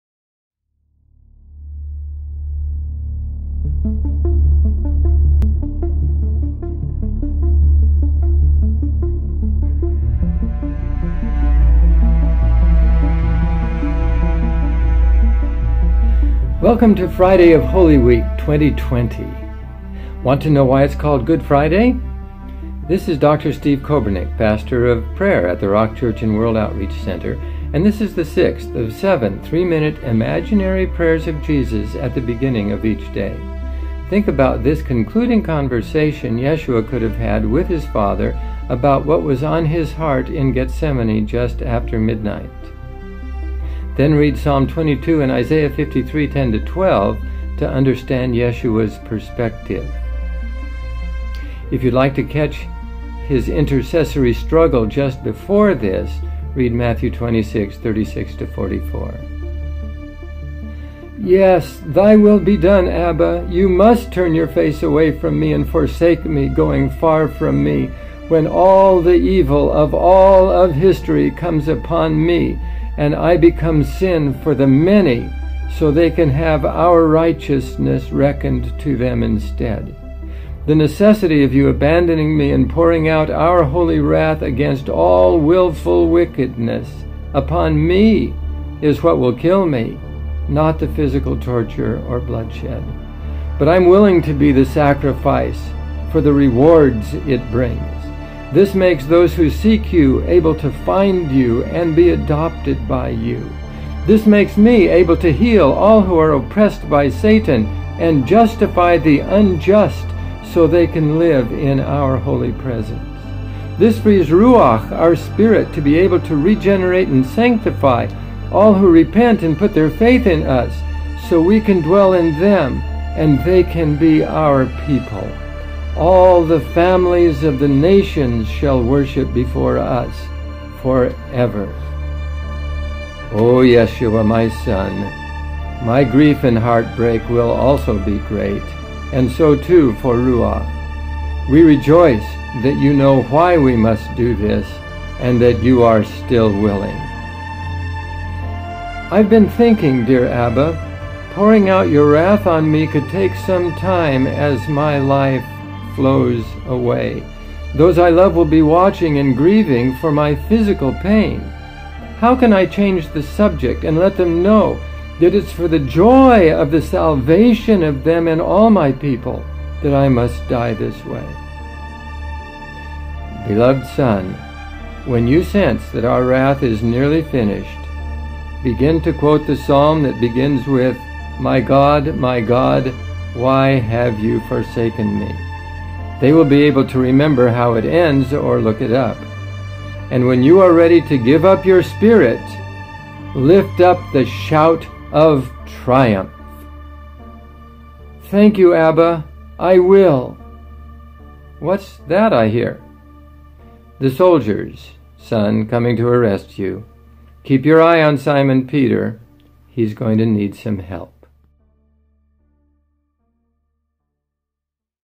Holy Week Day Six
Join Dr. Kobernik as he reads a prayer Jesus could have prayed with the Father during Holy Week leading up to His crucifixion, burial, and resurrection!

Visit: rock.church